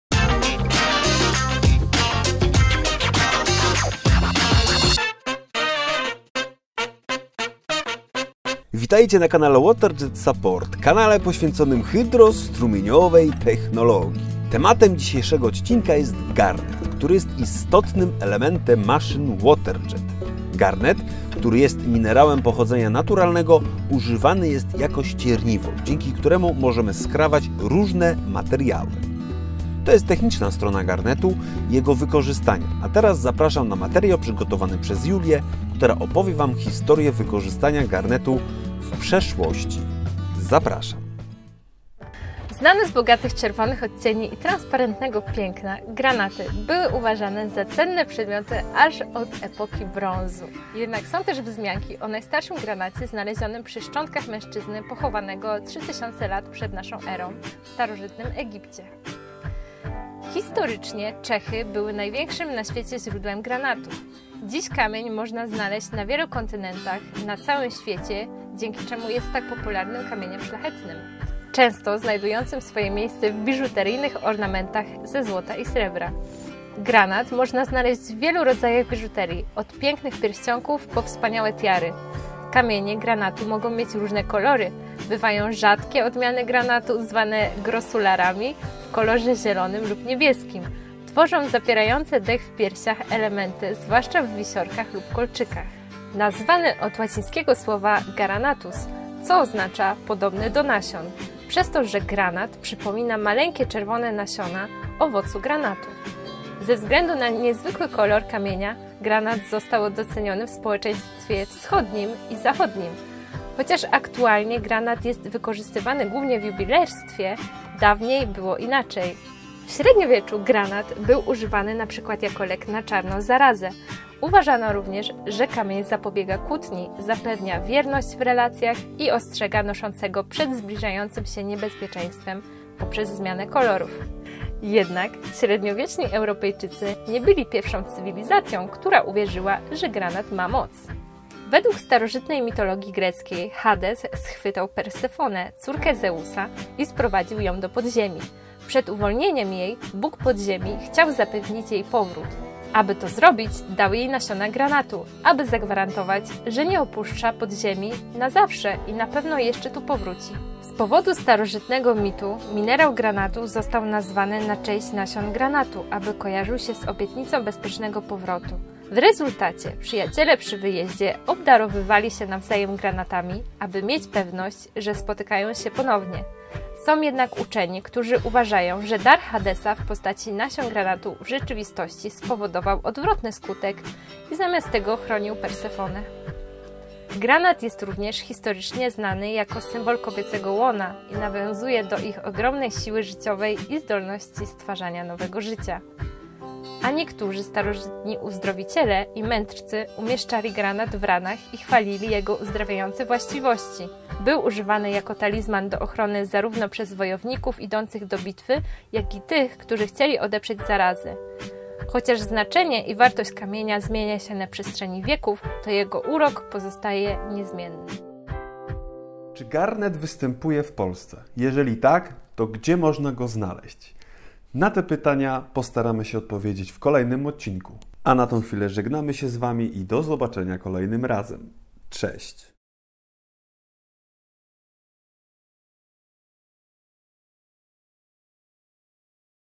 Ciekawostki o Garnet: Kluczowym Ścierniwie w Technologii Hydro-Ściernej
Masz pytania dotyczące technologii?
Chcesz umówić się na testy?

💎 W tym odcinku zapraszamy na dozę ciekawostek na temat garnetu. W świecie hydro-strumieniowej technologii używa się go jako ścierniwo, dzięki któremu możliwe jest precyzyjne cięcie twardych materiałów.

📜 A co mówi historia na ten temat? Wszystkie ciekawostki opowie Wam JULIA.

🌐 Źródłem informacji oraz zdjęć jest Internet. Dziękujemy wszystkim twórcom za udostępnione materiały!